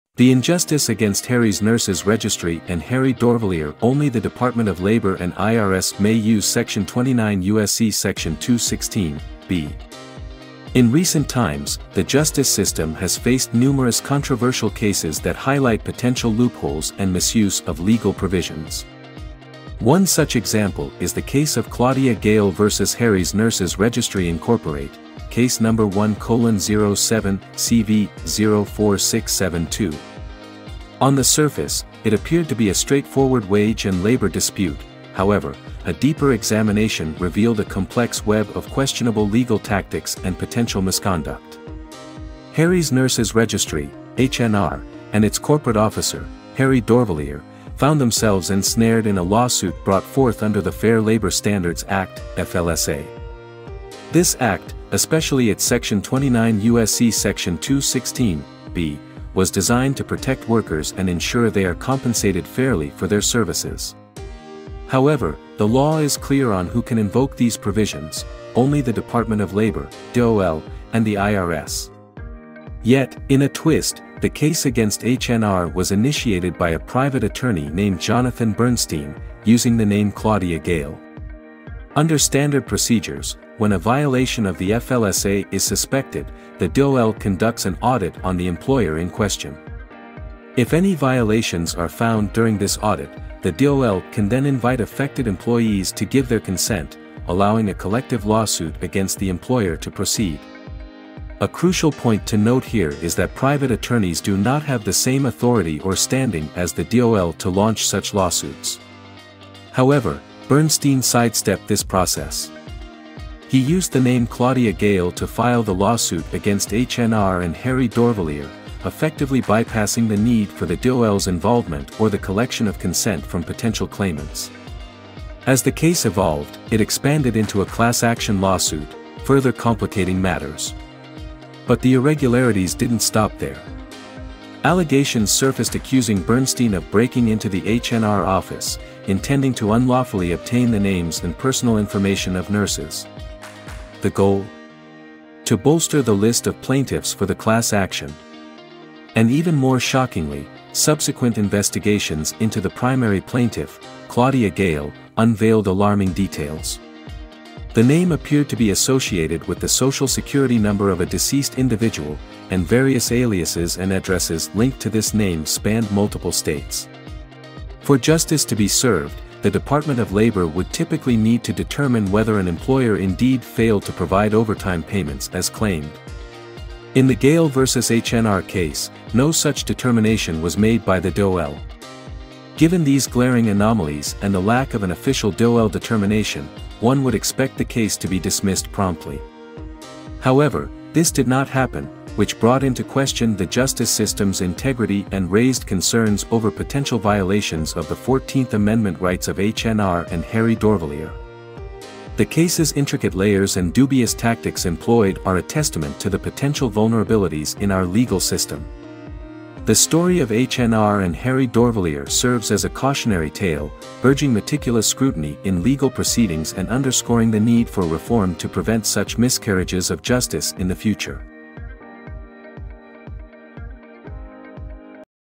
The Injustice against HNR and Harry Dorvilier: Only the DOL & IRS may use section 29 U.S.C. 216 (b)
In recent times, the justice system has faced numerous controversial cases that highlight potential loopholes and misuse of legal provisions. One such example is the case of Claudia Gayle v. Harry's Nurses Registry Inc. (Case number 1:07-cv-04672). On the surface, it appeared to be a straightforward wage and labor dispute; however, a deeper examination revealed a complex web of questionable legal tactics and potential misconduct.

Harry's Nurses Registry (HNR) and its corporate officer, Harry Dorvilier, found themselves ensnared in a lawsuit brought forth under the Fair Labor Standards Act (FLSA). This act, especially its section 29 U.S.C.A. 216 (b), was designed to protect workers and ensure they are compensated fairly for their services. However, the law is clear on who can invoke these provisions: only the Department of Labor (DOL) and the IRS.